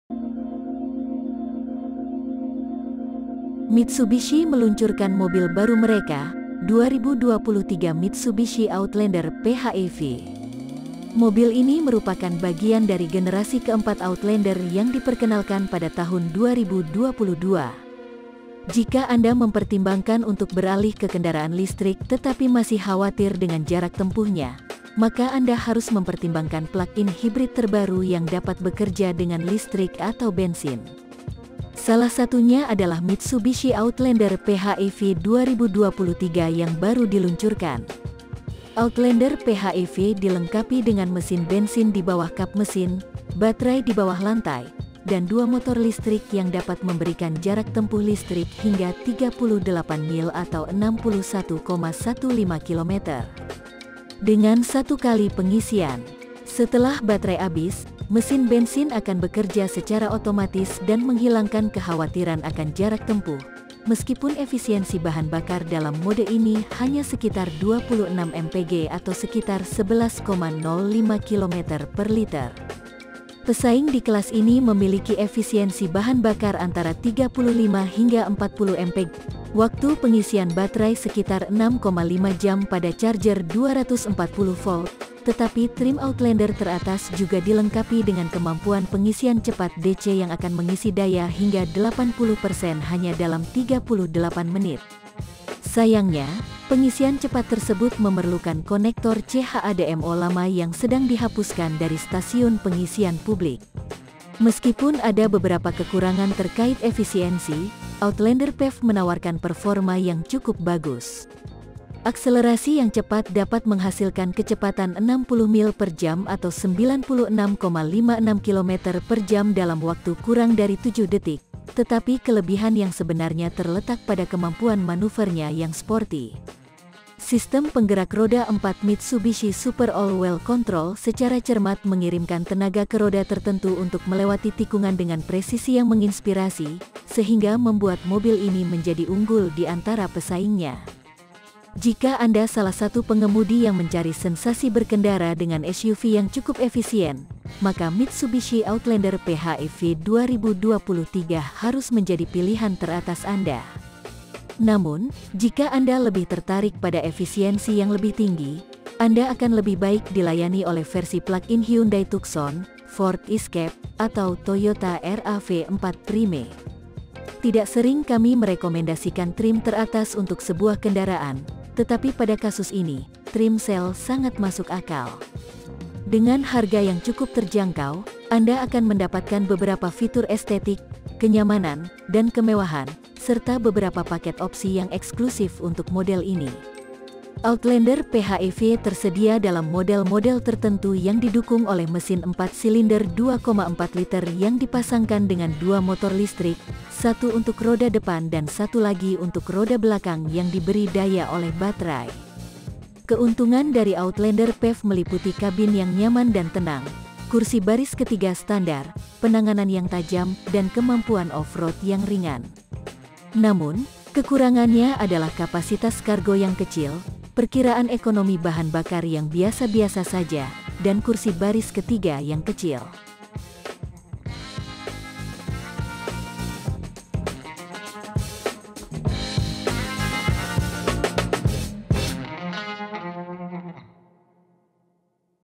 Review Mitsubishi Outlander PHEV 2023 Mobil SUV Plug In Hybrid Terbaik untuk Penggemar Sensasi Berke
Mitsubishi meluncurkan mobil baru mereka, 2023 Mitsubishi Outlander PHEV.
Mobil ini merupakan bagian dari generasi keempat Outlander yang diperkenalkan pada tahun 2022.
Jika Anda mempertimbangkan untuk beralih ke kendaraan listrik tetapi masih khawatir dengan jarak tempuhnya, maka Anda harus mempertimbangkan plug-in hybrid terbaru yang dapat bekerja dengan listrik atau bensin.
Salah satunya adalah Mitsubishi Outlander P H E V 2023 yang baru diluncurkan.